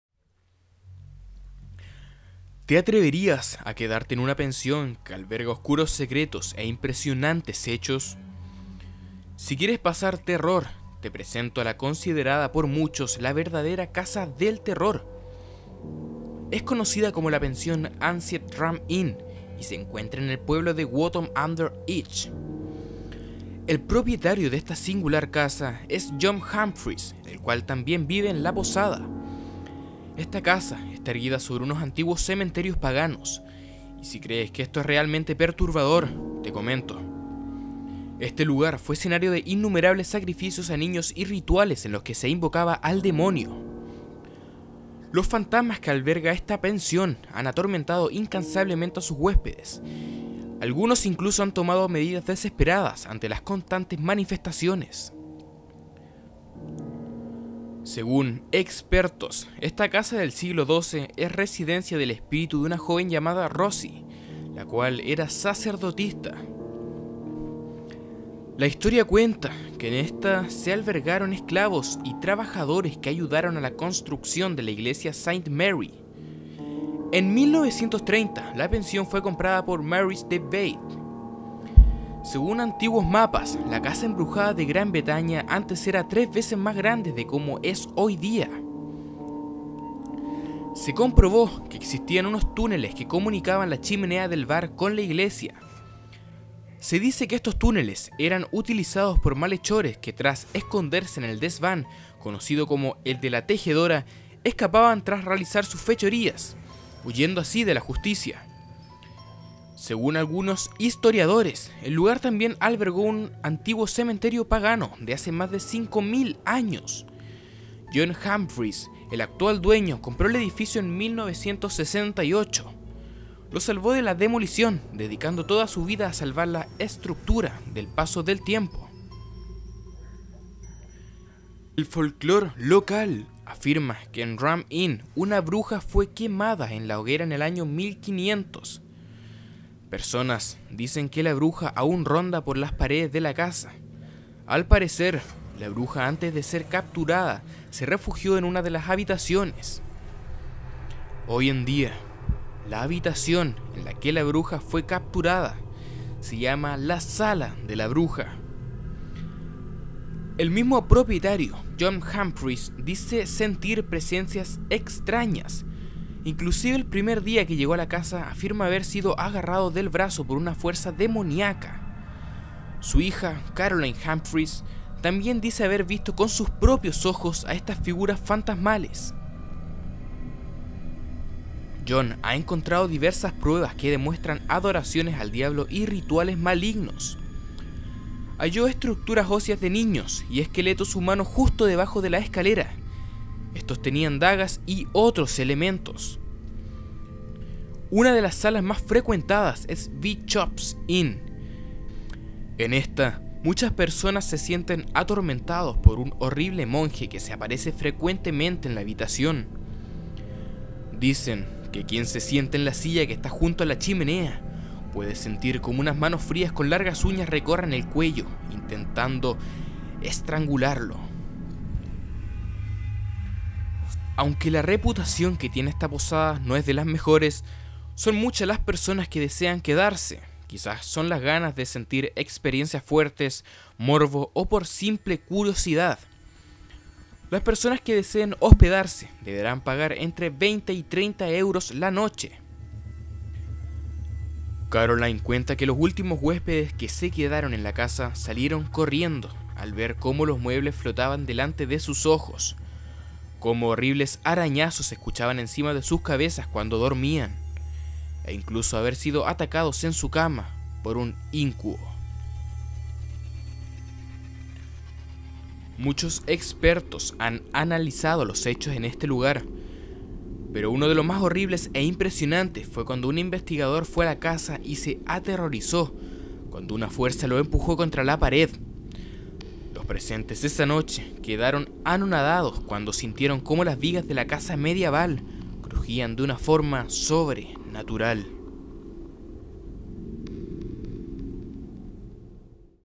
La Pensión mas Aterradora - TheCrissAlfa - Terror en 3, 2, 1
Nuevo video de dia domingo 8:00 (Hora Chilena).
Te atreverias a quedarte en esta espeluznante posada, deja en los comentarios tu opinión.

Twitter (TheCrissAlfa)